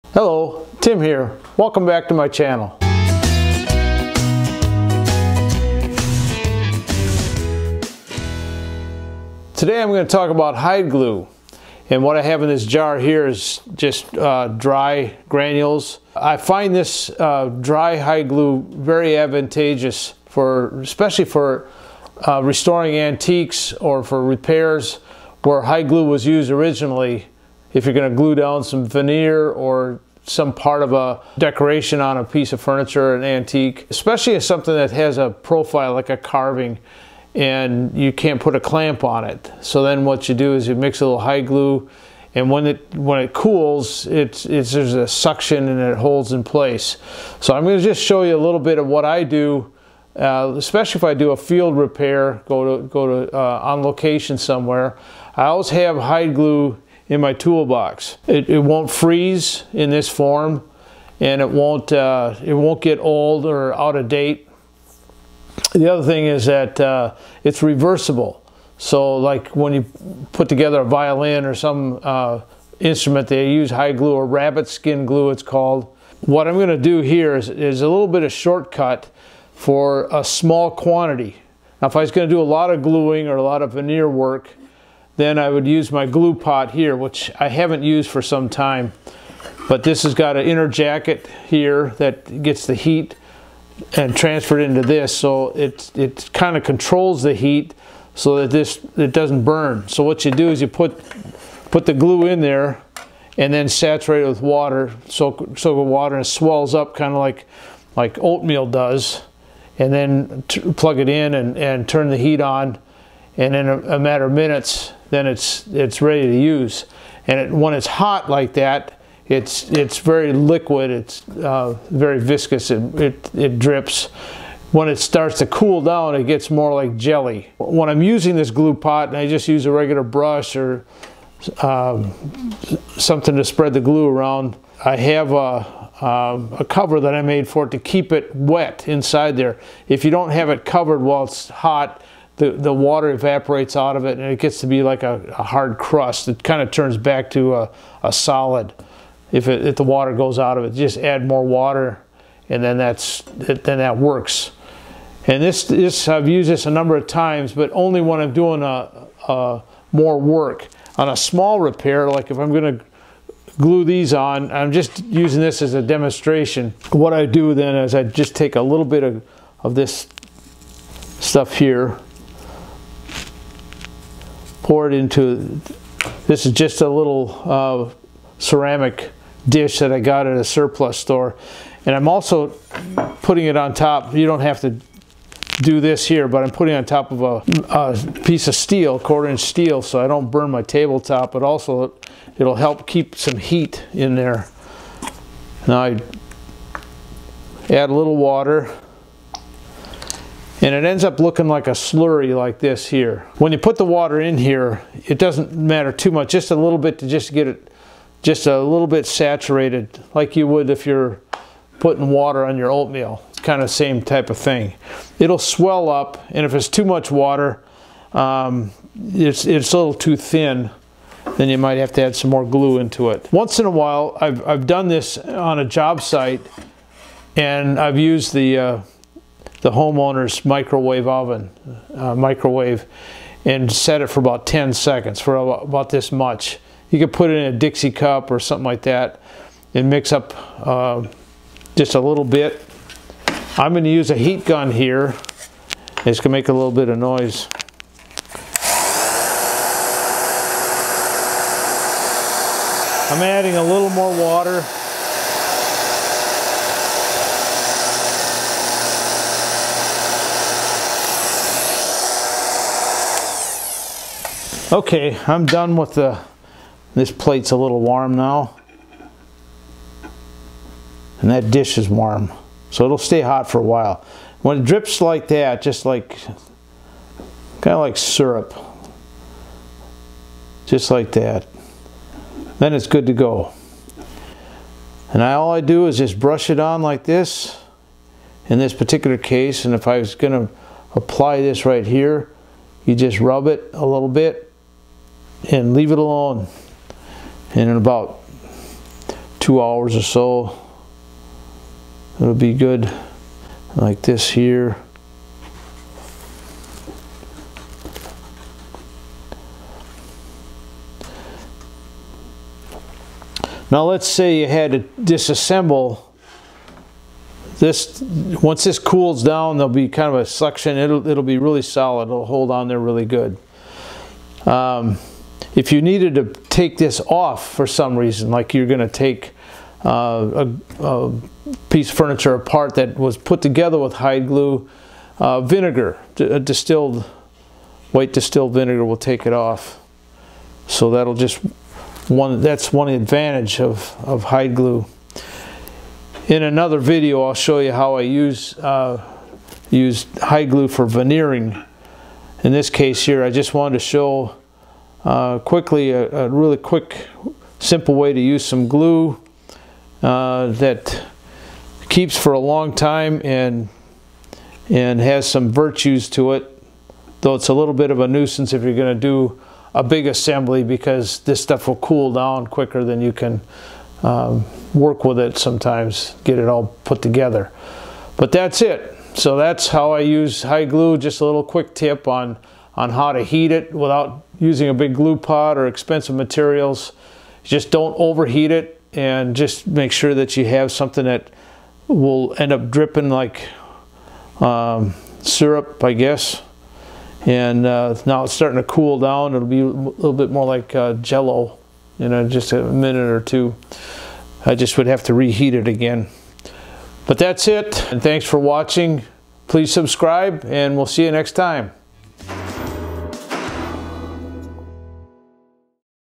How to Use Hide Glue
In this video I demonstrate how to use dry hide glue.